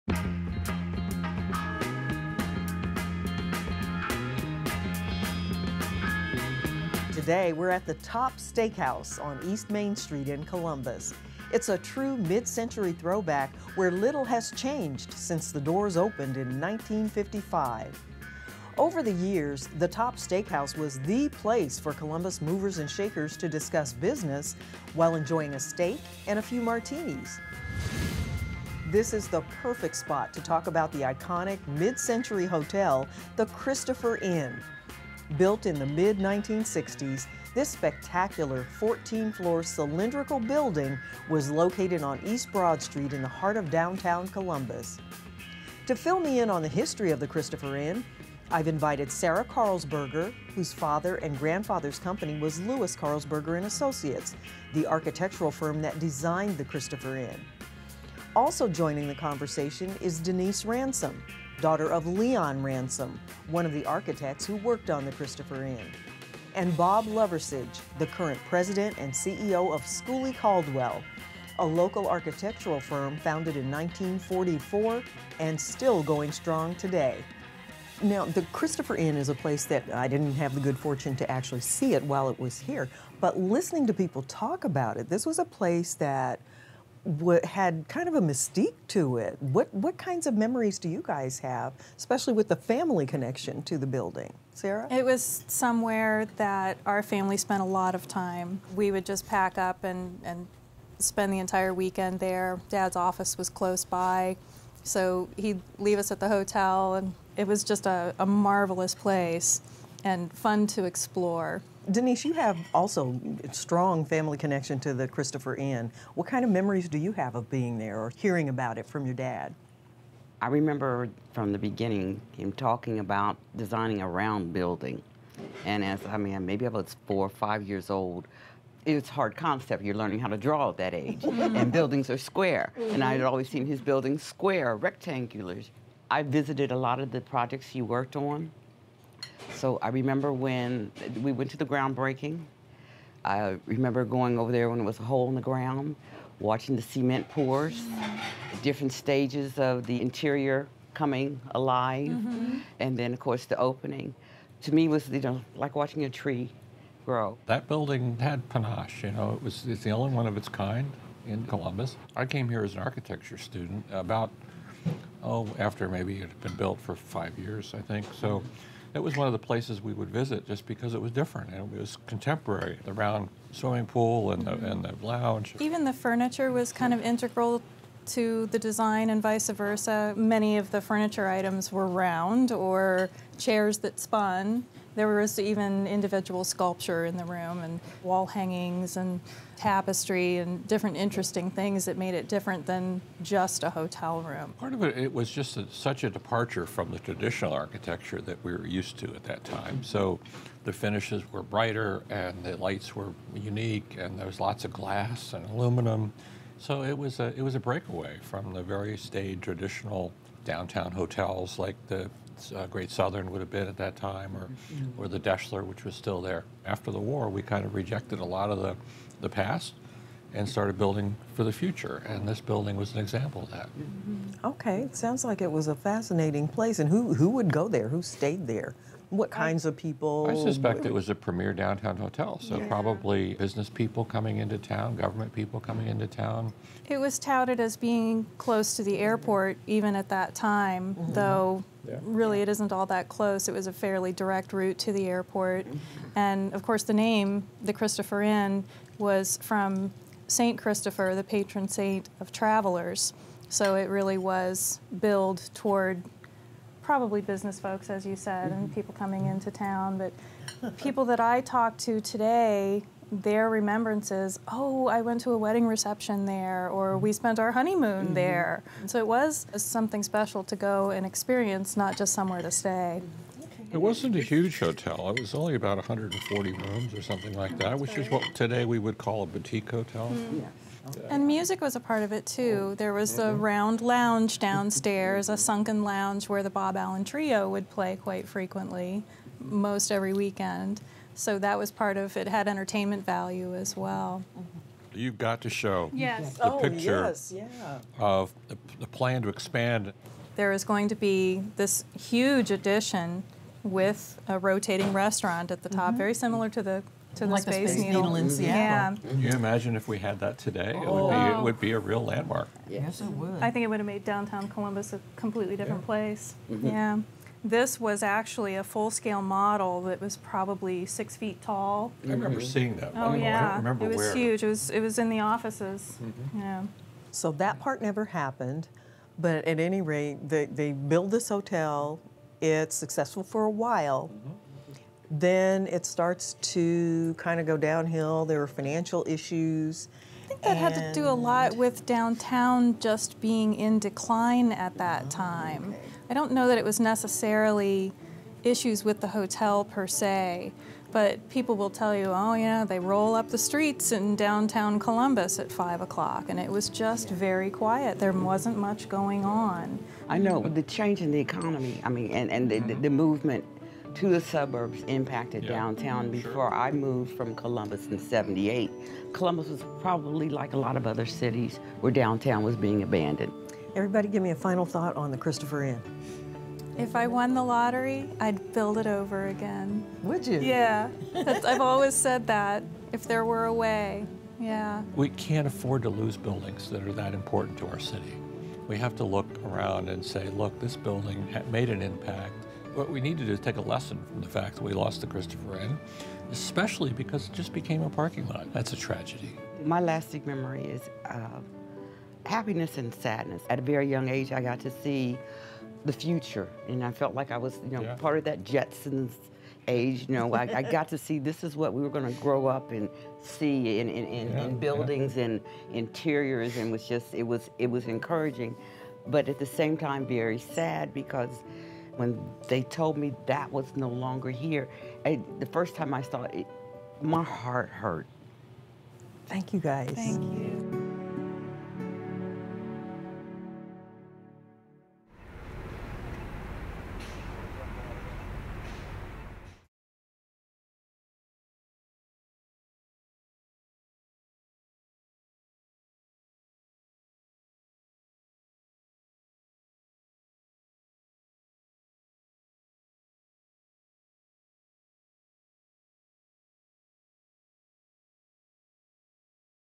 Columbus Neighborhoods: The Christopher Inn
Look! Up in the sky! It’s a cylinder! The Christopher Inn was a distinctive, prominent example of mid-century modern architecture on East Broad Street in downtown Columbus. Built in 1963 and demolished 25 years later, it’s still fondly regarded by those who remember it in the city’s skyline.

Photos courtesy of: Sarah Karlsberger, Grandview Heights Public Library, Columbus Dispatch, Columbus Metropolitan Library, Denise Ransom, Ohio History Connection